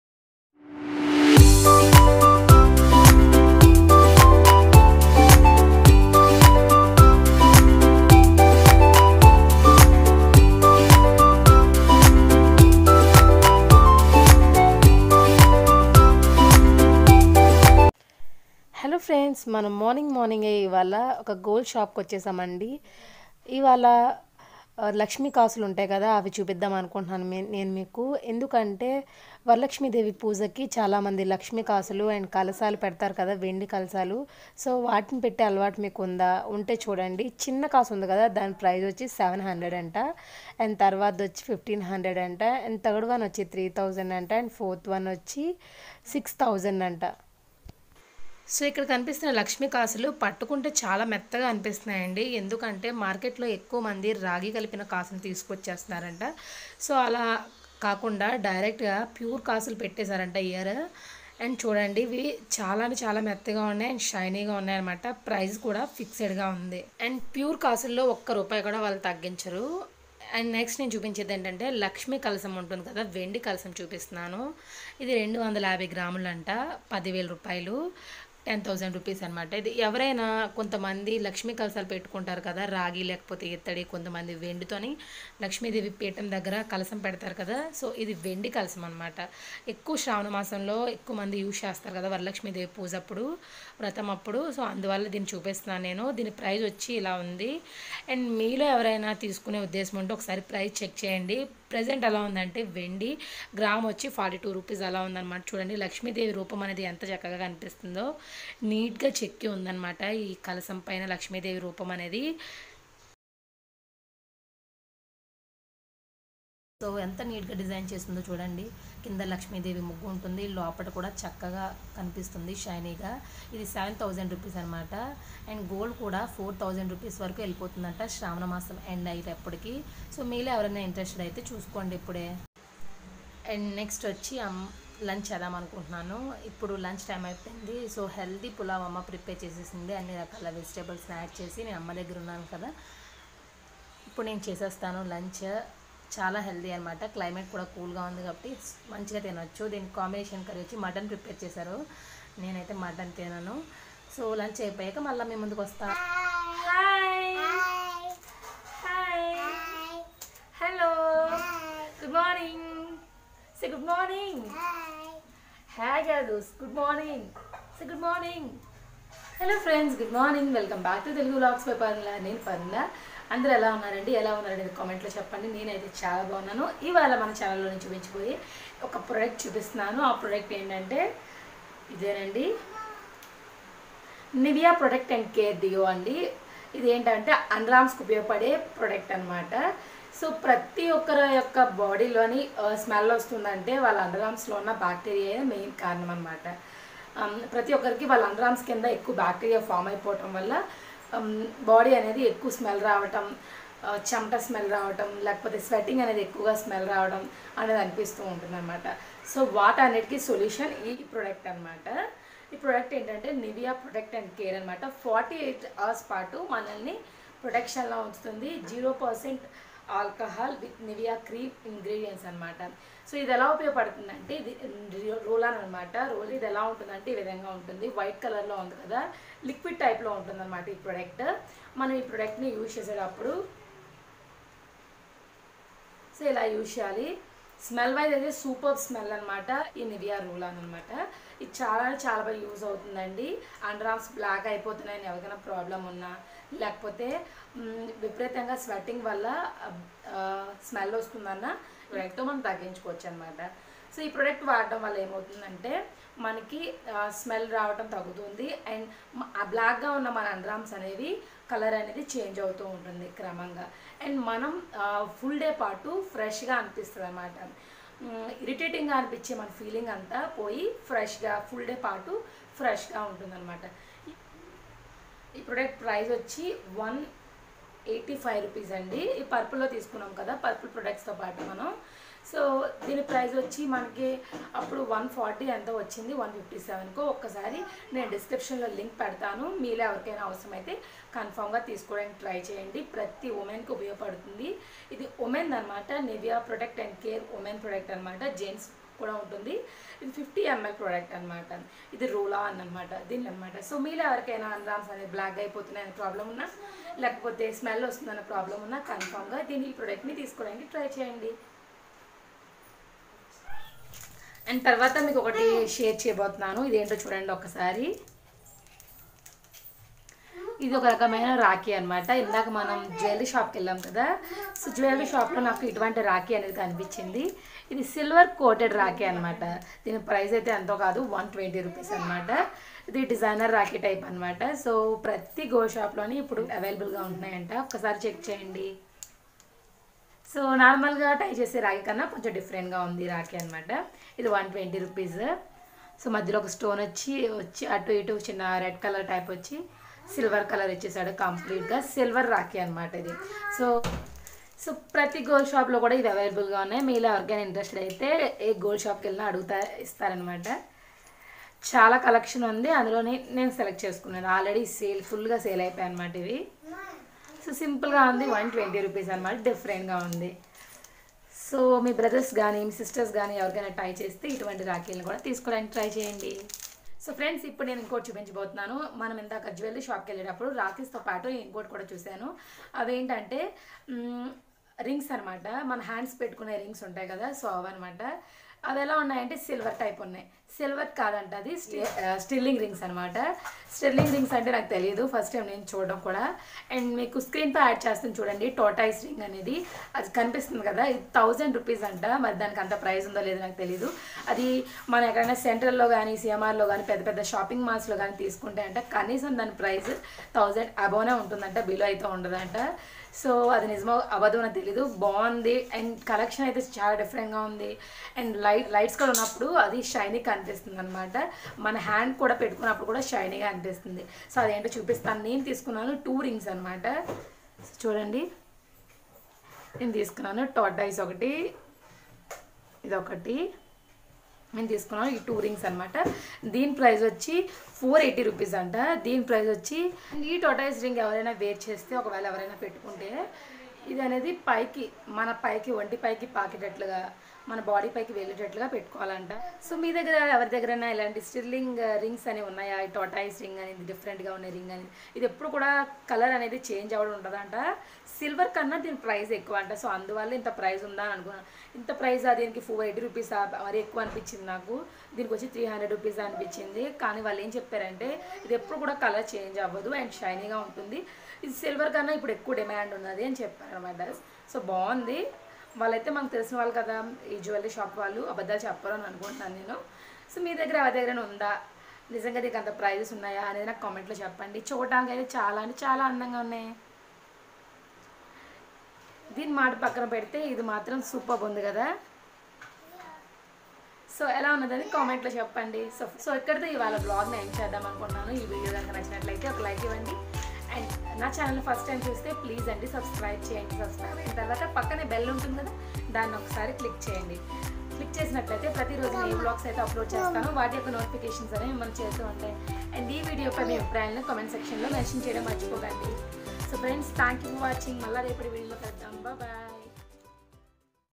శ్రావణమాసంలో లక్ష్మీకాసు,లక్ష్మీకలశం రేటు /underarmsలో నలుపు,దుర్వాసన తగ్గించేది/సిల్వర్ కోటెడ్ రాఖీ
శ్రావణమాసంలో
అమ్మవారికి పెట్టే బంగారులక్ష్మీ కాసు,వెండి లక్ష్మీ కలశం  రేటు ఎలా ఉందో చూడండి
వెండి కన్న ఎక్కువ రేటు ఉన్న Sterling rings
 prices...